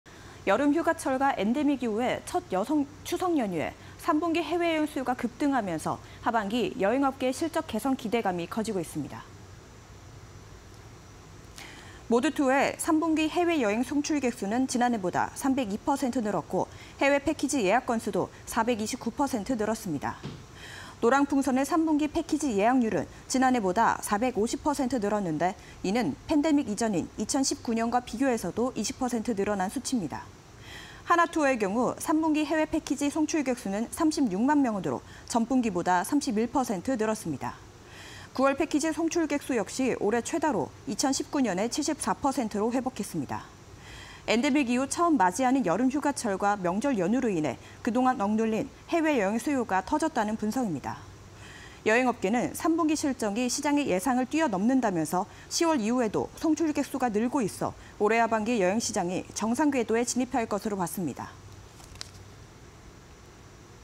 3분기 해외여행 수요 폭풍 성장… 실적 개선 신호탄 (20231006)
# 3분기 해외여행 수요 폭풍 성장… 실적 개선 신호탄